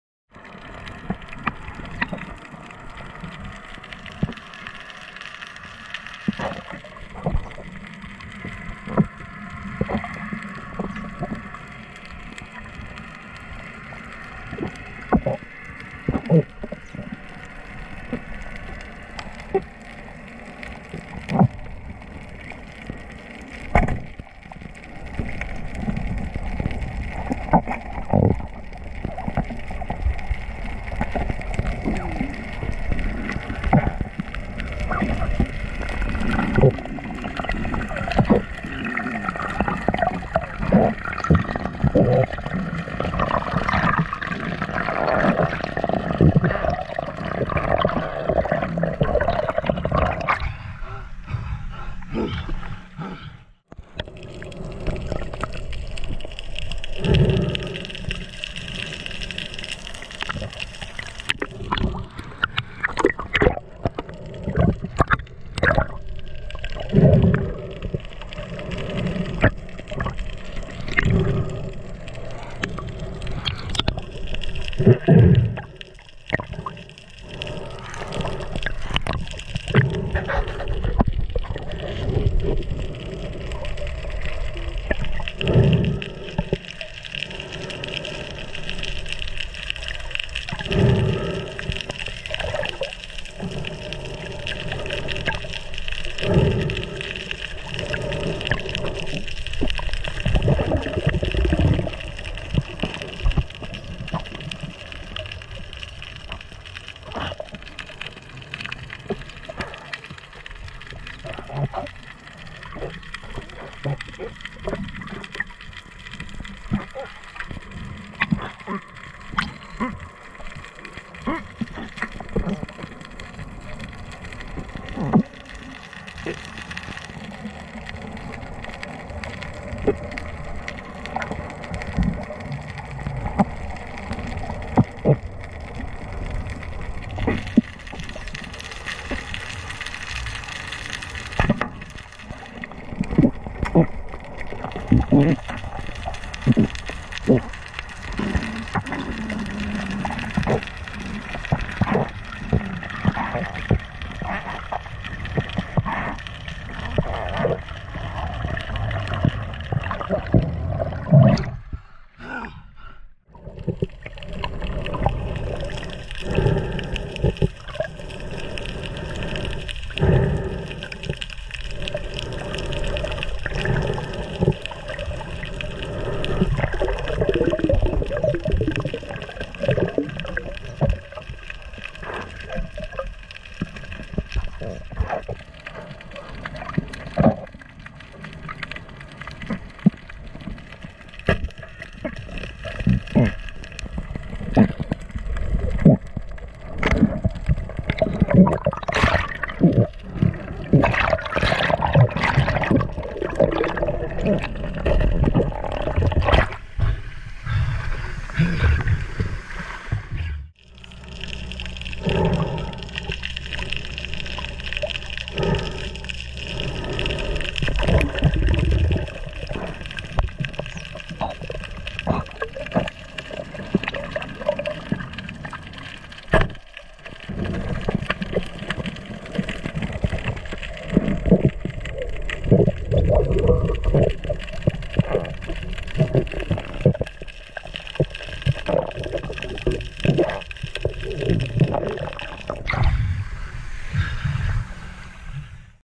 Texas Spearfishing Mangrove Snapper 7-25-2021
Spearfishing for mangrove snapper on Texas oil rig Gulf of Mexico while freediving. multiple dives. Was a great day with some good visibility. Mangroves were hanging around 60-70ft. Below 75ft was bad visibility.